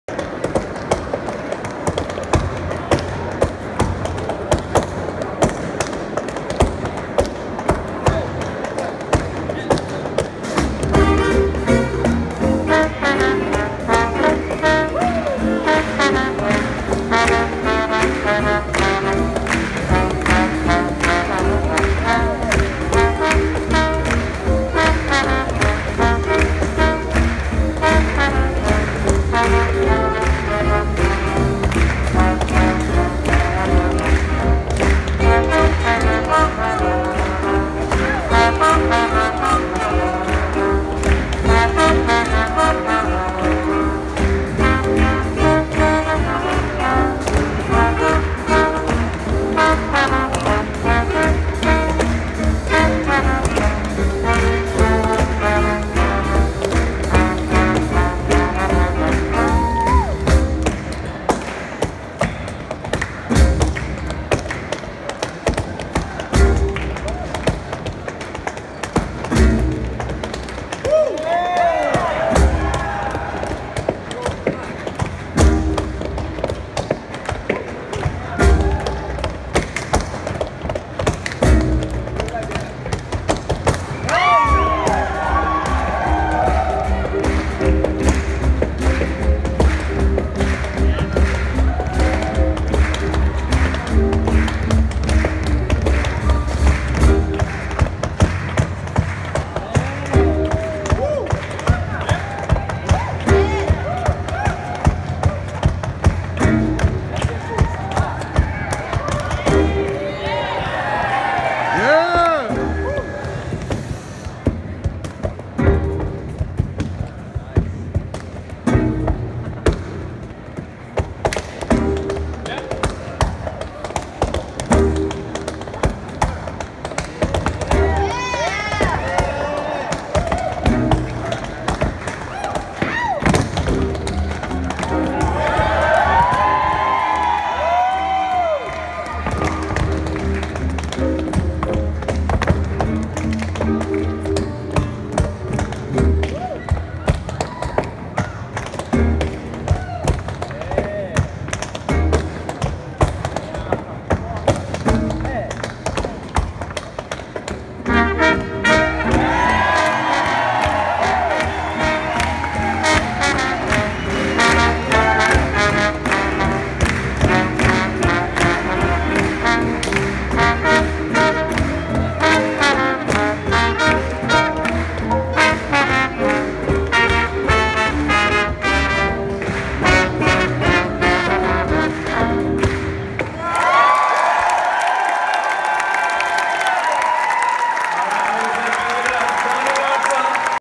Chase Festival 2019 Tap - Daniel Larrson, Sakarias Larrson & the Hornsgatan Ramblers
Daniel and Sakarias Larrson performing with the Hornsgatan Ramblers at Chase Festival 2019